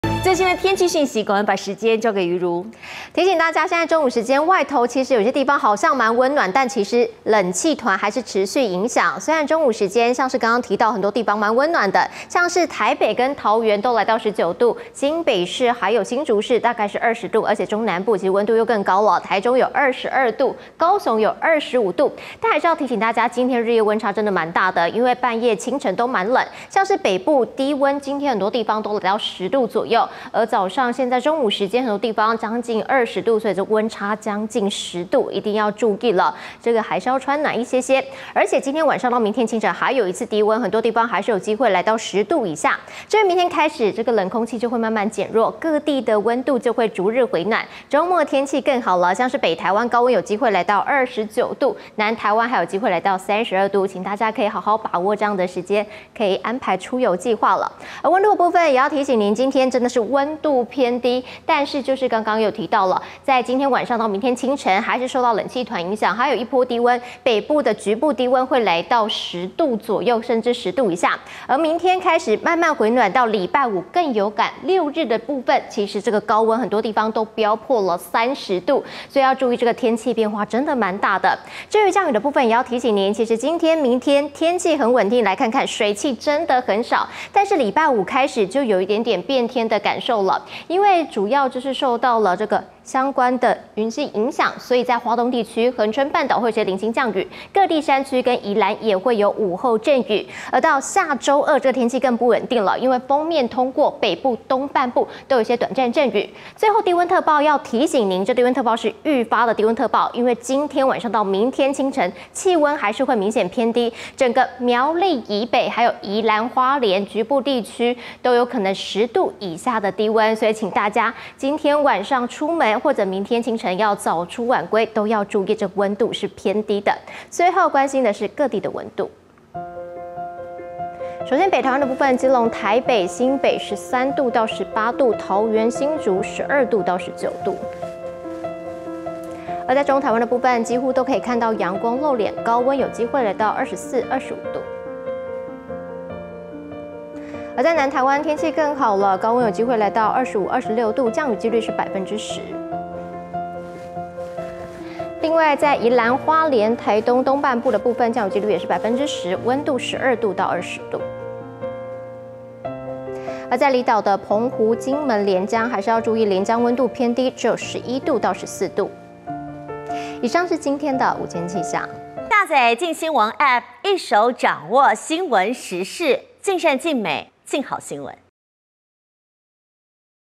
今日春分！明起氣溫漸升 週末高溫飆30度｜鏡新聞
更多天氣資訊，交給氣象主播周瑜茹。